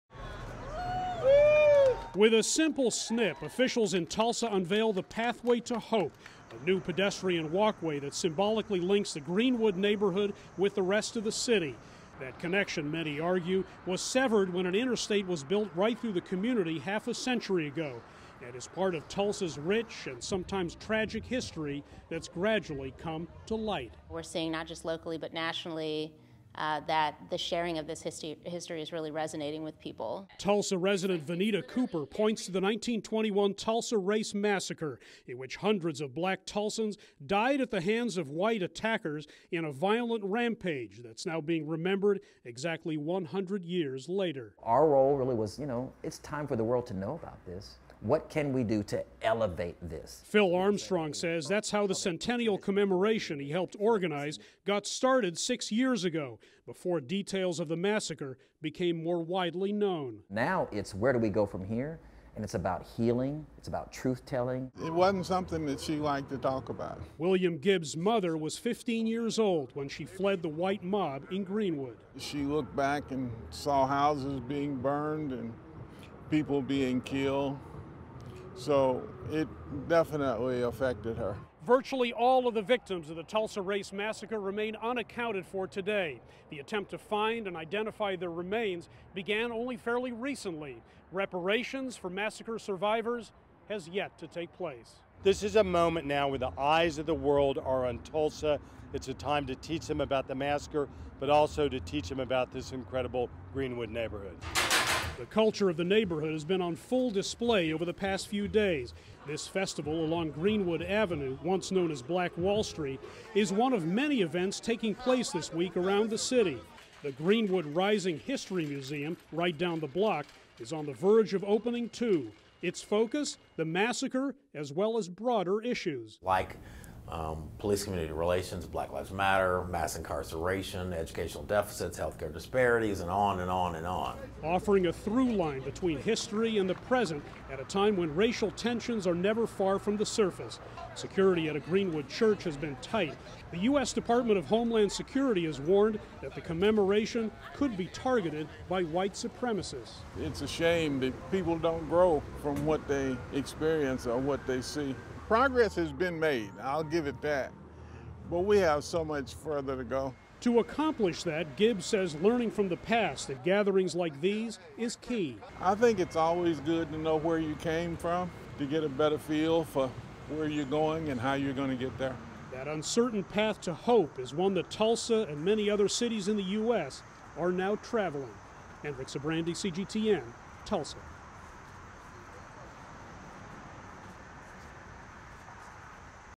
Tulsa Race Massacre remembered 100 years after the terror
100 years ago, the arrest of a Black man for allegedly assaulting a White woman sparked perhaps the worst episode of racial violence in U.S. history.

Hundreds of Blacks were killed in the 1921 Tulsa Race Massacre, an event that was suppressed by the city of Tulsa, Oklahoma, for many years. The event is being remembered there now.

CGTN’s Hendrik Sybrandy reports.